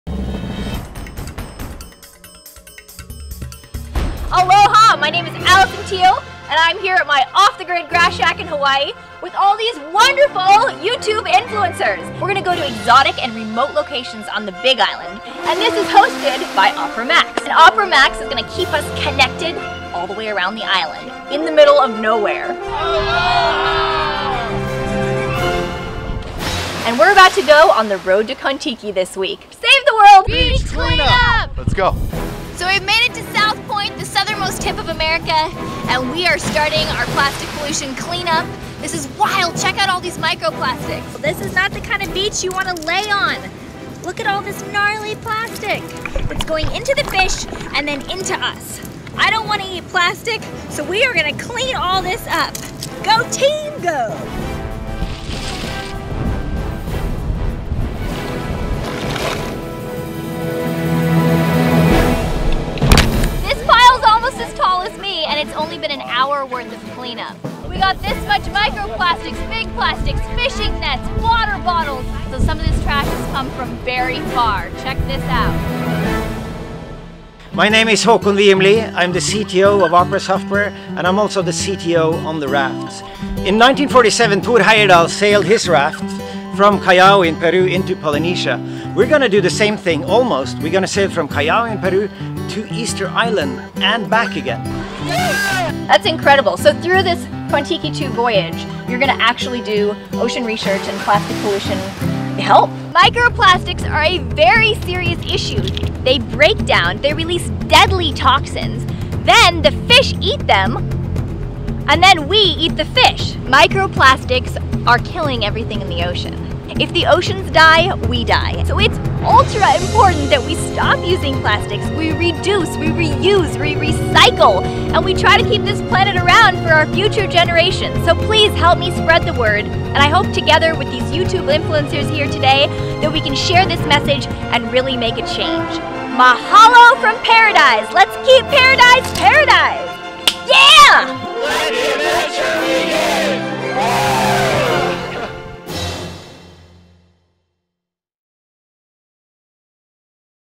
Road To Kontiki
Once upon a time there was a Norwegian explorer by the name of Thor Heyerdahl who was an adventurer who decided to take on a big journey from Peru to the Eastern Island. Heyerdahl decided that he would build a raft that he named Kon-Tiki and would take it on this grand expedition across the South Pacific Ocean.

This is a moment inspired another Norwegian man to take on the same adventure. Håkon Wium Lie who is CTO at @operabrowser and is the founder of Cascading Styles Sheet or known as CSS is getting ready to take the same expedition Heyerdahl took.

The adventure is beginning at my grass shack in Hawaii on RoadToKontiki where YouTube stars joined to change the world leading up to Kontiki2

AND most importantly, since the Kontiki mission will focus on plastic pollution research, I took the crew to do a local clean up, focusing our message for the week on reduce, reuse, recycle!